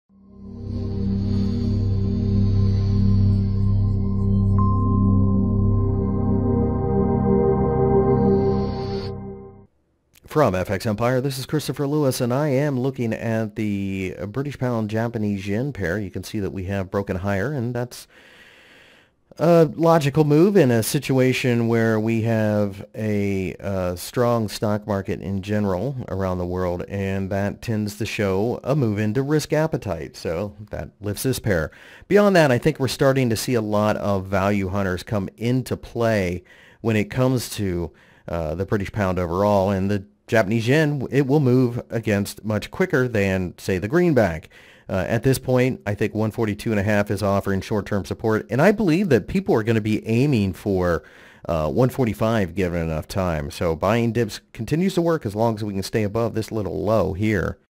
GBP/JPY Technical Analysis for August 27, 2018 by FXEmpire.com
The British pound rallied against the Japanese yen during trading on Friday, to round off a week of positive momentum. For the full article go - GBP/JPY Forecast August 27, 2018, Technical Analysis.Find more information about Forex and Commodities News Technical Analysis Fundamental Analysis and Brokers on FX Empire